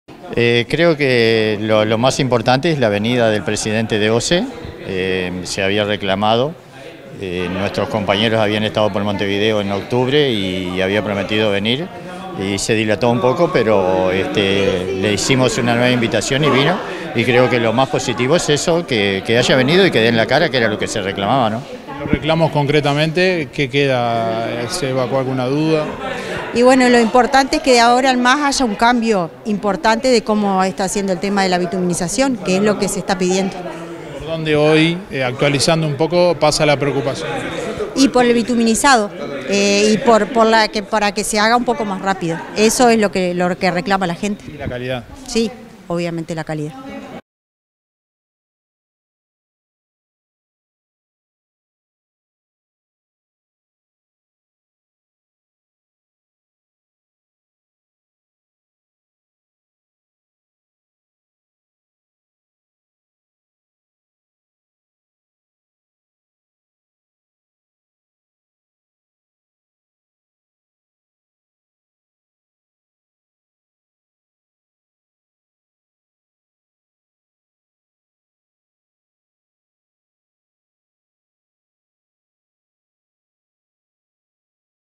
Reinaldo Bouvier y María Laura Alles | Centro Comercial Juan Lacaze
Los integrantes de Centro Comercial de Juan Lacaze Reinaldo Bouvier y María Laura Alles valoraron la reunión de forma positiva.

Director y Conductor: Darío Izaguirre